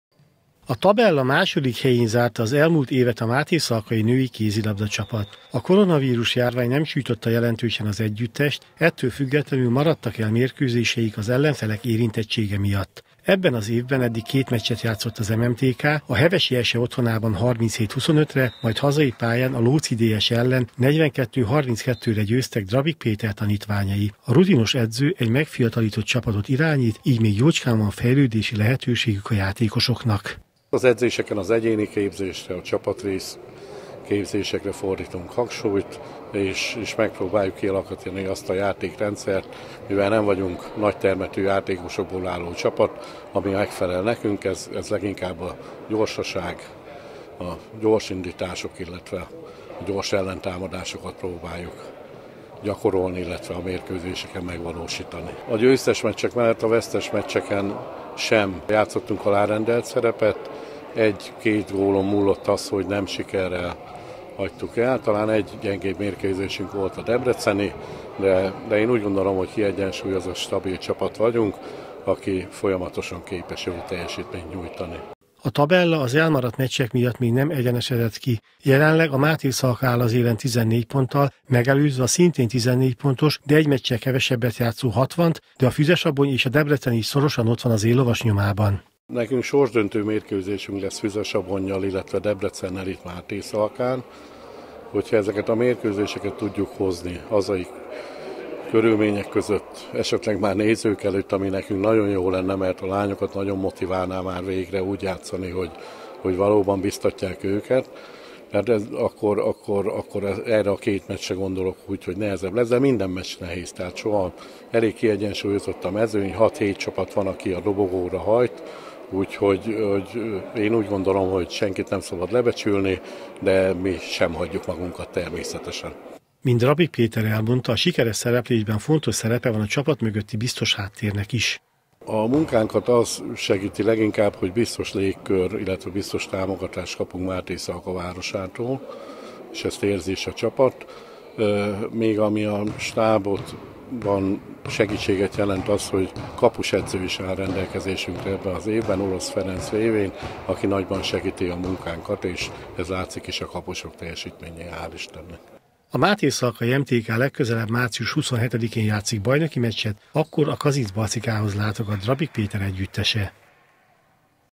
Élen az MMTK
Hét győzelemmel és három vereséggel vezeti a női kézilabda NB II Észak-keleti csoportjának tabelláját a Mátészalkai MTK. A Drabik Péter által irányított együttes legközelebb a Kazincbarcika otthonában lép pályára.